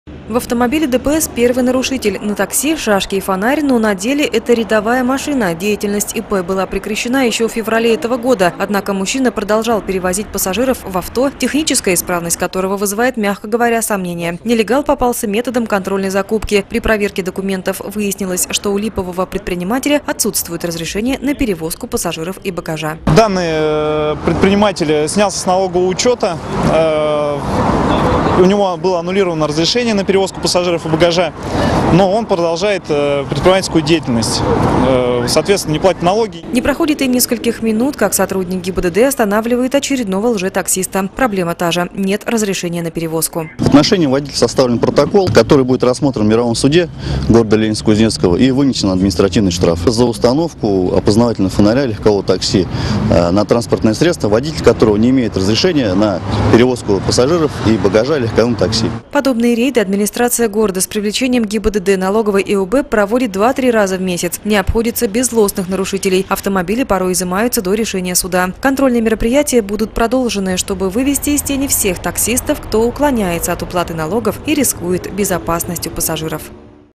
Нелегалам - бой
Городские власти борются с таксистами-нелегалами. В Ленинске-Кузнецком прошел очередной рейд с участием сотрудников отдела промышленности, транспорта и связи, ГИБДД и налоговой инспекции. Результаты рейда в следующем сюжете.